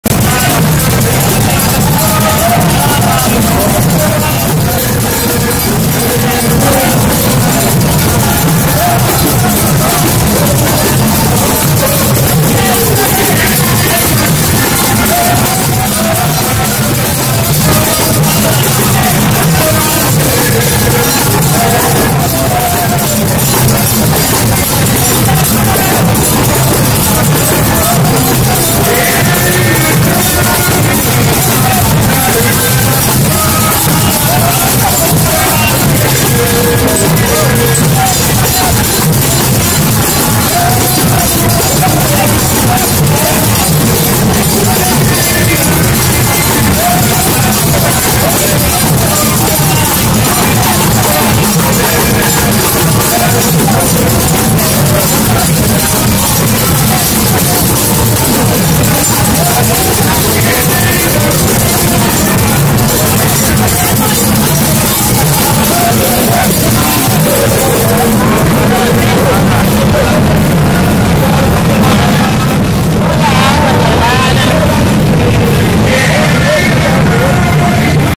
Washing of the Steps Festival in Salvador Brazil 2014.
This is a "religious" festival in Salvador, Brazil, that is the second largest celebration next to Carnival. It includes a procession of the traditional Bahian women in white dresses who carry flowers in vases to the Church of Senhor do Bonfim.